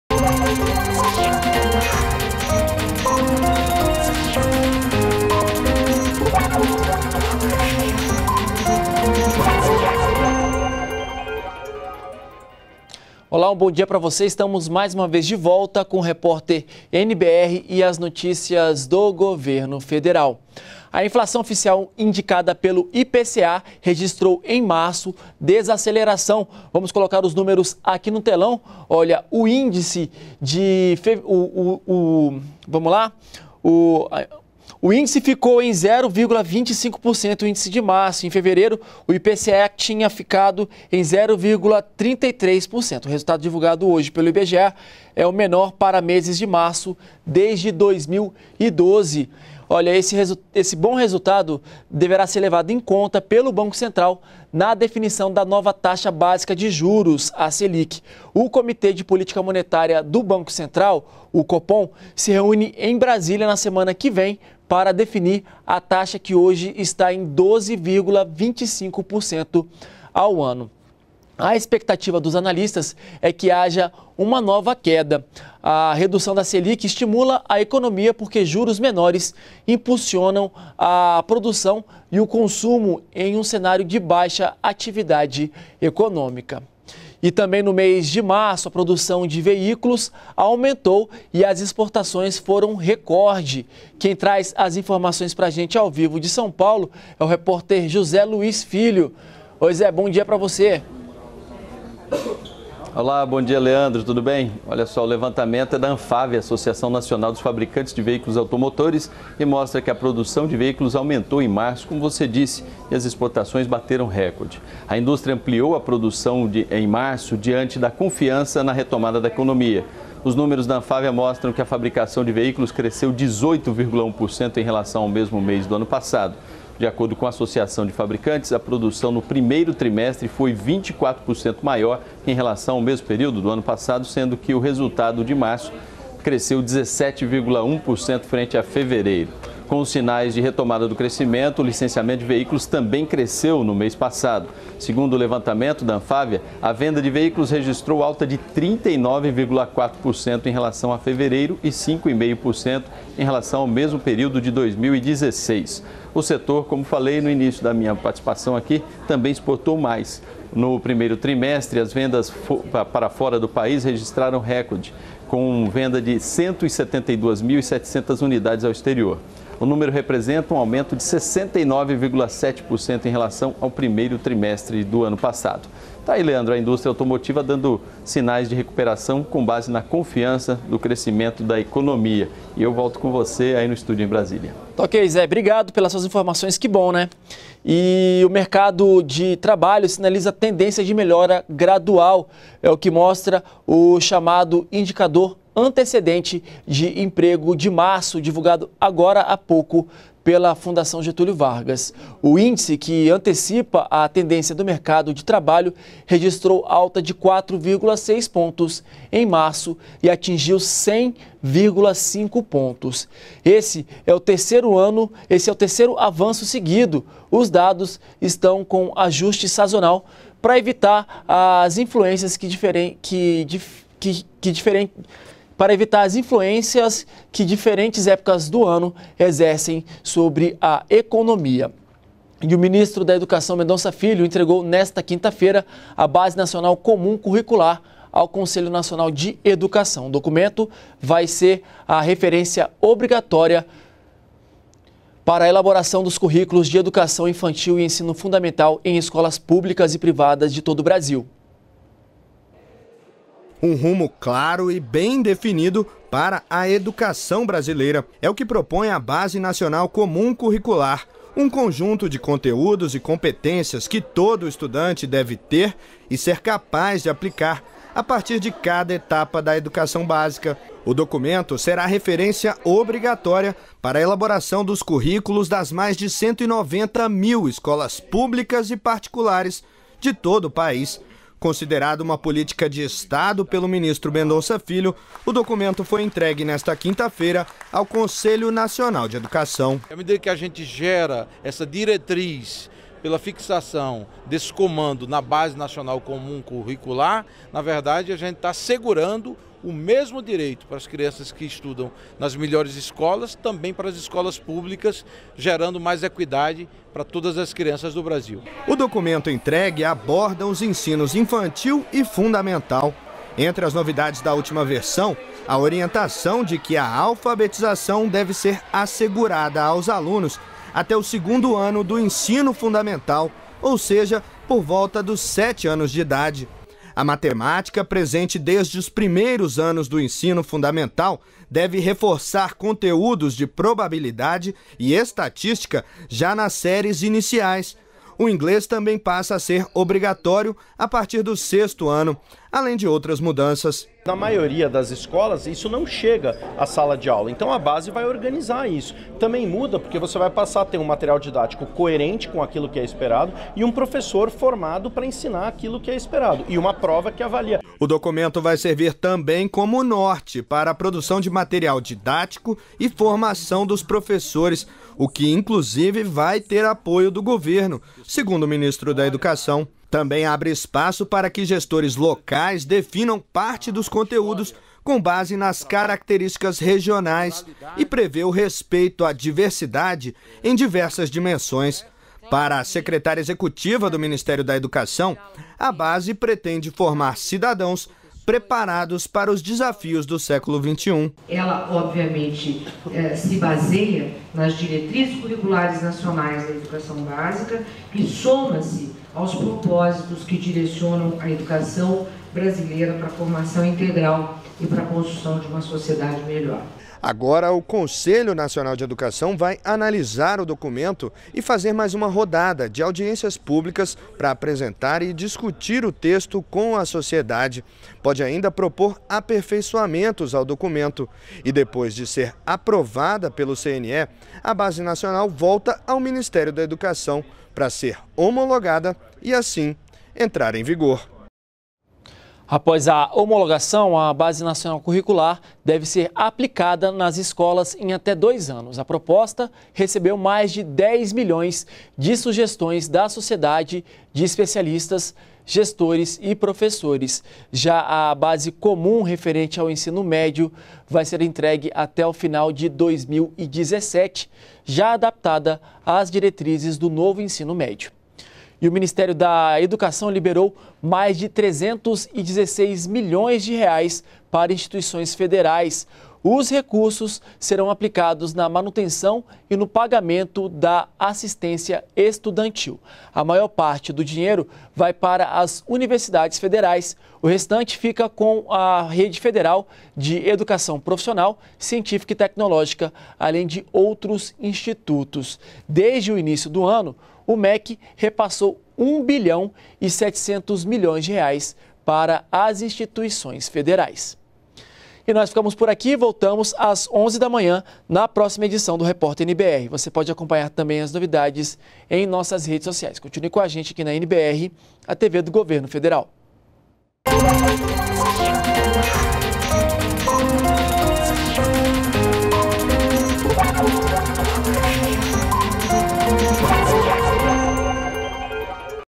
Mercado de trabalho sinaliza tendência de melhora gradual
REPÓRTER NBR 10H - 07.04.17: A inflação oficial, indicada pelo IPCA, registrou desaceleração em março ficando em 0,25%. Resultado divulgado nesta sexta-feira (7) pelo IBGE é o menor para meses de março desde 2012. E ainda: O Indicador Antecedente de Emprego de março mostra que o mercado de trabalho sinaliza tendência de melhora gradual.